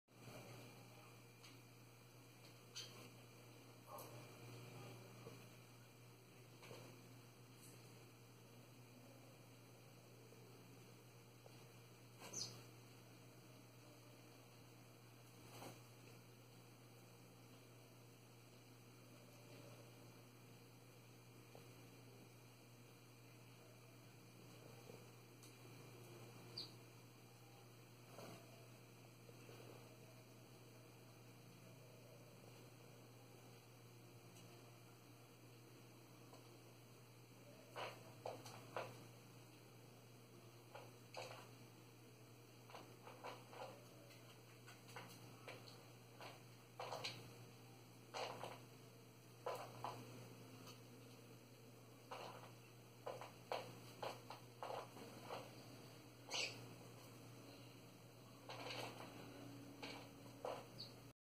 Pardais comendo a ração do meu cachorro
Enquanto o meu PISCHER dorme os pardais fazem a festa almoçando a ração dele.

Dormiu perdeu playboy haha.